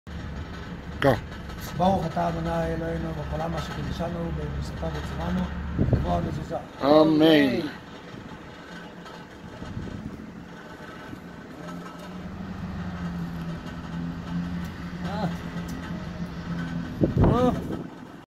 Affixing the Mezuzah on the doors of our new mechanic shop. קביעת מזוזות בעסק החדש
Affixing the mezuzah

According to halakha, the mezuzah should be placed on the right side of the door or doorpost, in the upper third of the doorpost (i.e., approximately shoulder height), within approximately 3 inches (8 cm) of the doorway opening.